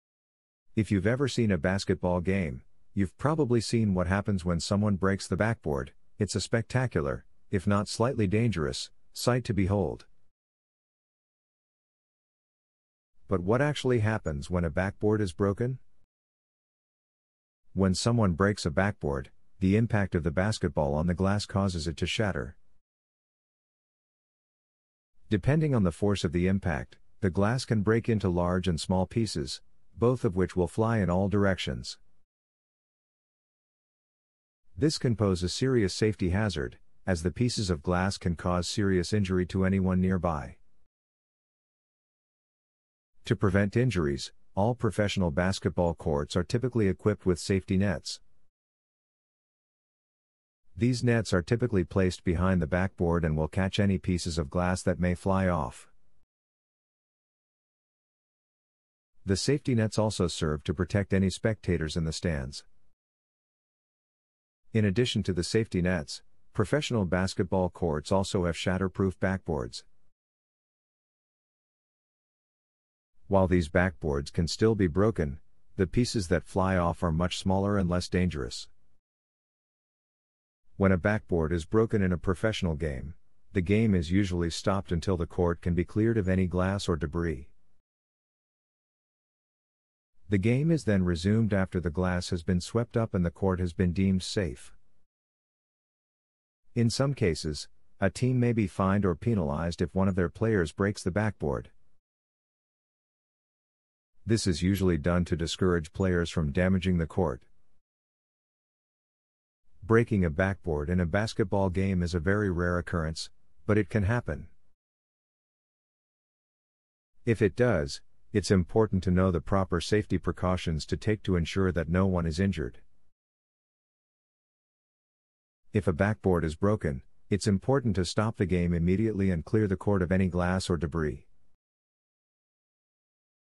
What Happens If Someone Breaks The Basketball Backboard?
Have you ever wondered what happens if someone breaks a basketball backboard? The answer might surprise you. A broken backboard can cause a great deal of damage to both the court and the player who is responsible for the break. In this video, we'll take a look at what happens when a backboard breaks, the measures taken to repair it, and the consequences that can come with a backboard break.

When a backboard breaks, it is usually due to a player dunking the ball with too much force. The force of the dunk can cause the glass to shatter, the metal rim to bend, and the backboard to crack. The broken glass and metal can become dangerous projectiles that could injure other players or spectators in the arena. In order to prevent any harm from occurring, the game must be stopped and the court cleared of all debris.

After the court has been cleared of all broken glass, the backboard is inspected to decide if it can be repaired or if it needs to be replaced. In most cases, the broken backboard can be repaired by replacing the broken pieces and reinforcing the frame. The cost of the repair depends on the extent of the damage, but the cost can range from hundreds to thousands of dollars.

In addition to the cost of repair, a broken backboard can also have legal consequences. Depending on the league and the severity of the damage, the player responsible could be suspended or even face criminal charges. It is also important to note that any damage done to the court or other players could result in a hefty fine.

Breaking a backboard is a serious offense, and all players should be aware of the consequences that come with it. It is important to take proper safety precautions when playing basketball and to avoid making dunks that could result in a backboard break. The cost of repairs and the legal implications of a backboard break can be expensive and stressful, so it is important to be aware of the risks involved in playing the game.